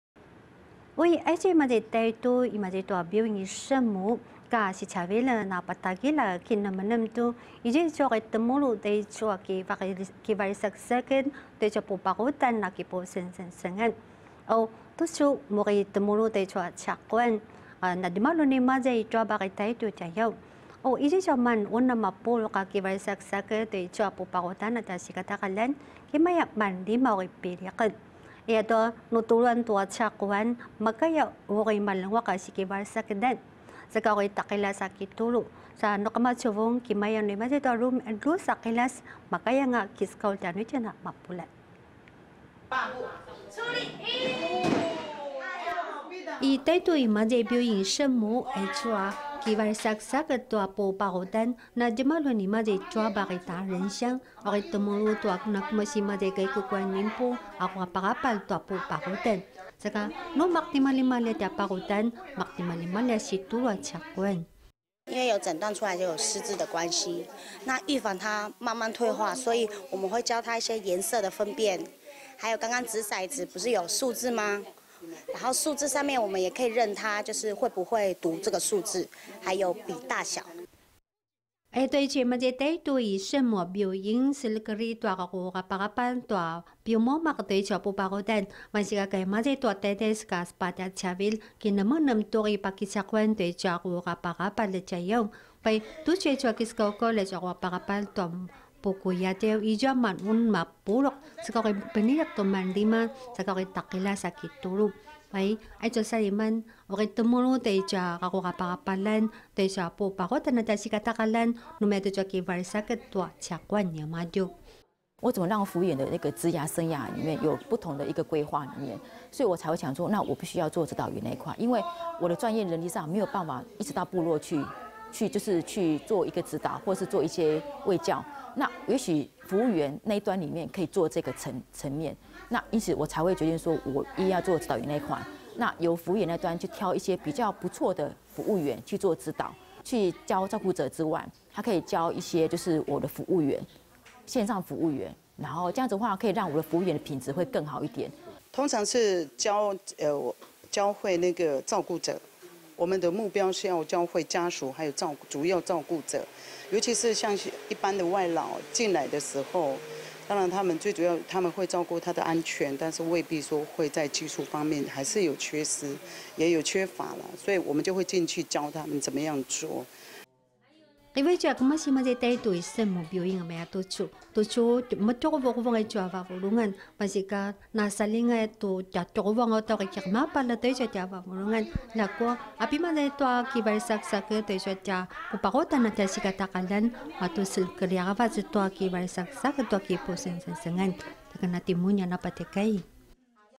居家照顧技術指導員培訓 提升專業能力 2016-03-26 Paiwan TITV 原視族語新聞
台東聖母醫院長期照顧相關人員，來到達仁鄉土坂部落，在居家照顧技術指導員，以及物理治療師的協助下，教導外籍看護一些基本及正確的照護觀念，並針對不同的照顧個案，設計不同的課程指導。

台東聖母醫院因為人力不足，104年開始籌備「居家照顧技術指導員」的培訓，並從60位的居服員挑選5位，在今年1月開始，為期1個月的受訓，2月開始主要協助居服員、照顧者或是外籍看護，指導他們在照護上的觀念。

台東聖母醫院表示，人口老化的關係，受照顧者需求不斷的增加，但是在照顧者人力方面，還是相當不足。